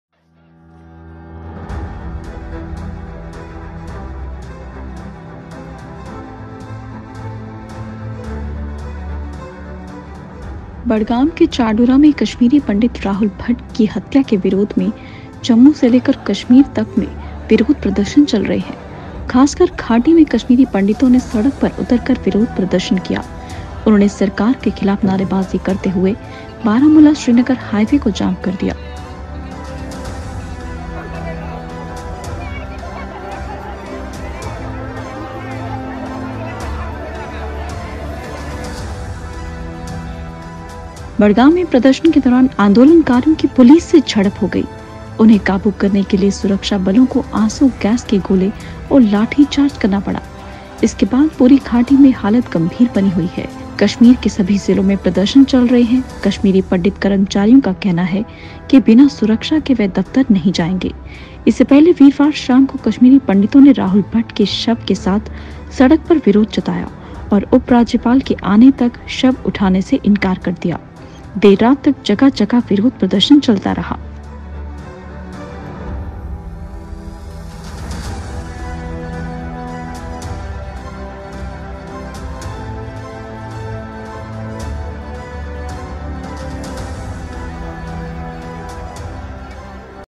कश्मीरी पंडितों का फूटा गुस्सा, जम्मू से लेकर कश्मीर तक प्रदर्शन, पुलिस से धक्का-मुक्की
कश्मीरी पंडितों का फूटा गुस्सा, जम्मू से लेकर कश्मीर तक प्रदर्शन, पुलिस से धक्का-मुक्की के बाद लाठीचार्ज